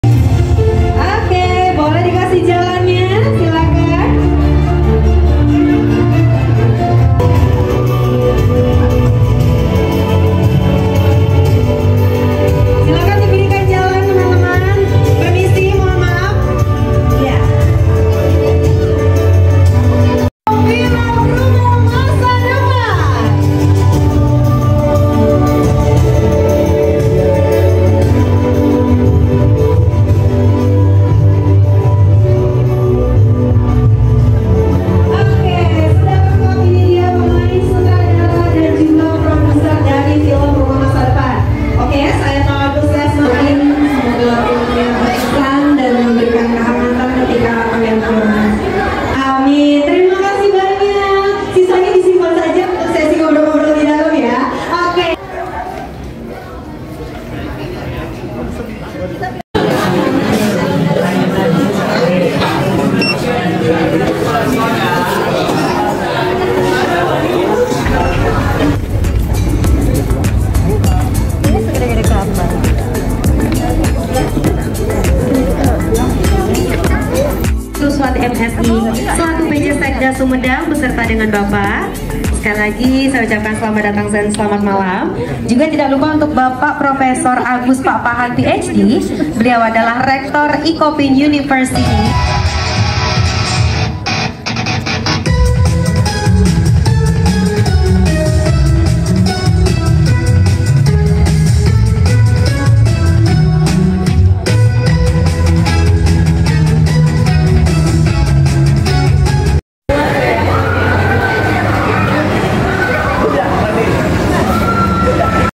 FILM RUMAH MASA DEPAN GALA PREMIERE 2 DESEMBER 2023 | Banyak Aktor Aktris 😍 | Spot Videography
Bberapa para pemain

Fedi Nuril
Laura Basuki
Widyawati
Cok Simbara